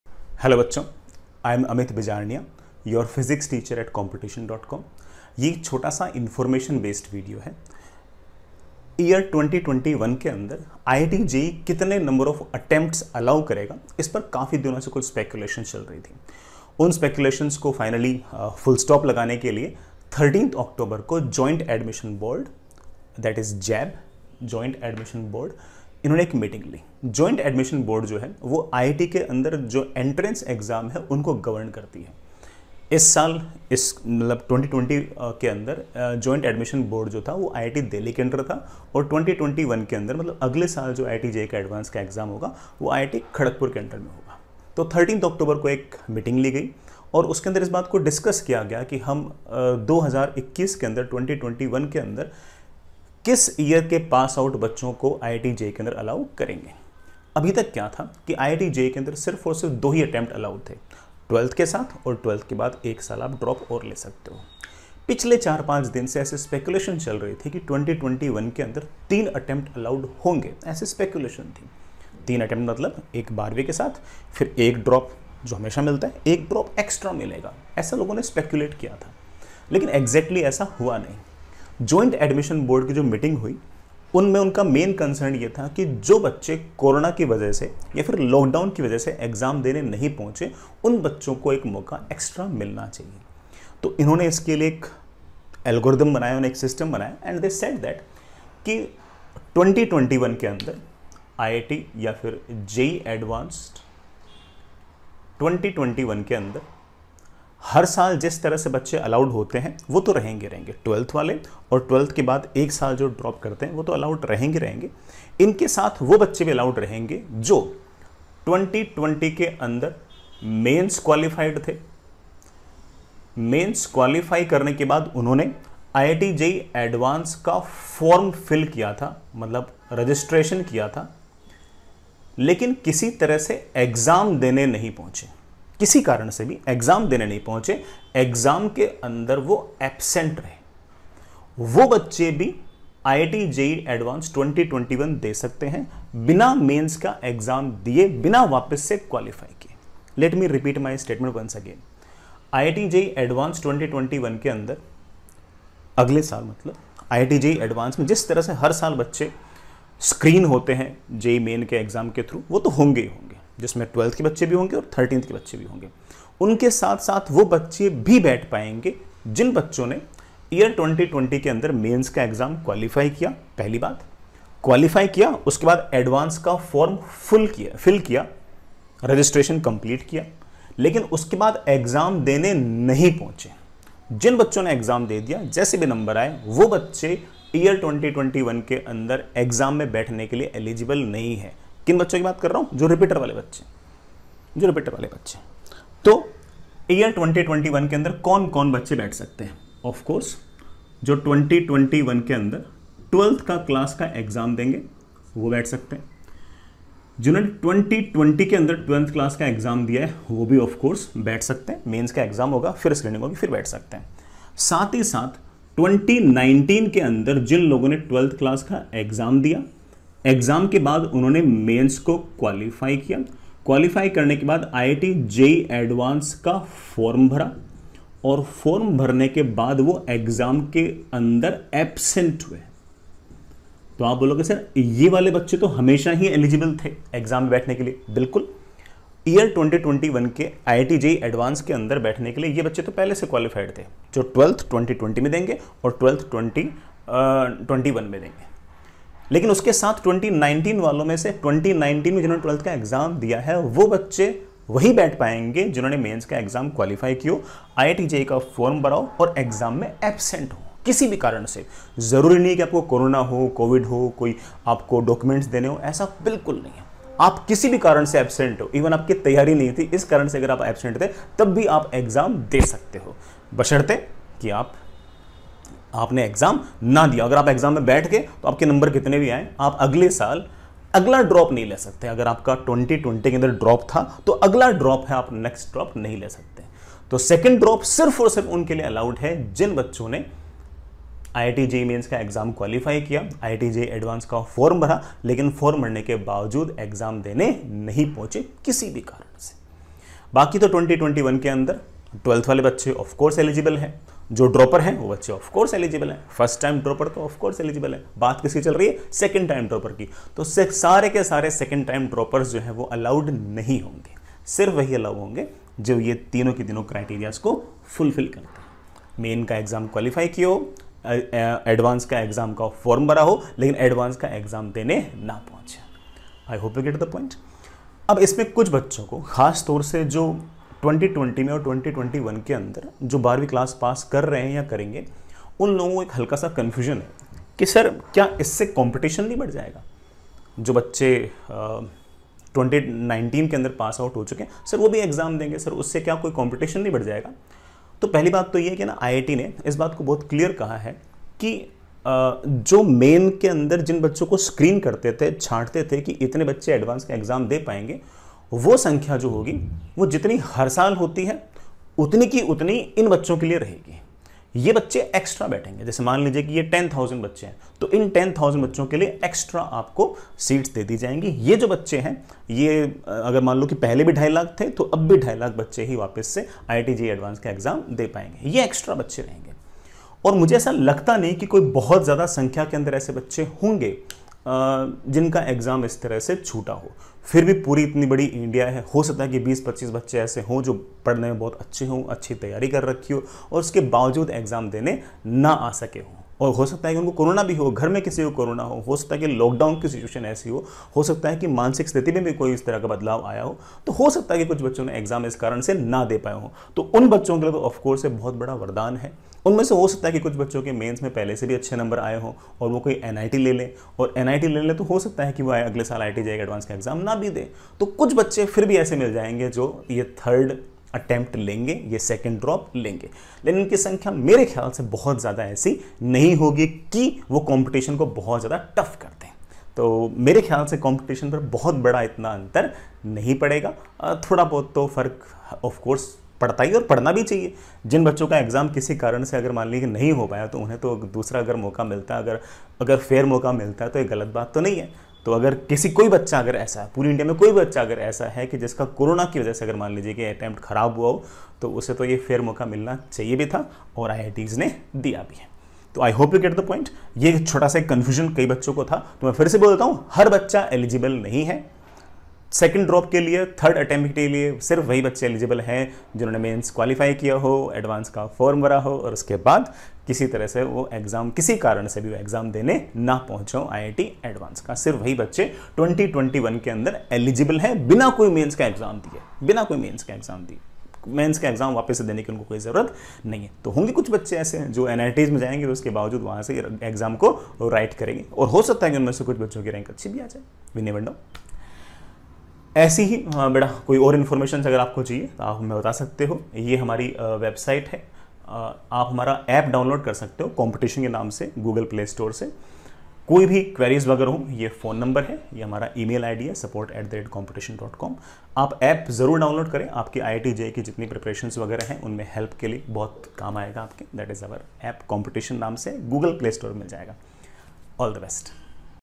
3rd attempt in JEE advanced for absentees| 2nd drop in IIT advanced| JAB meeting| one more chance
IITJEE main and Advanced examination has cleared its take on 3rd attempt for IIT JEE Advanced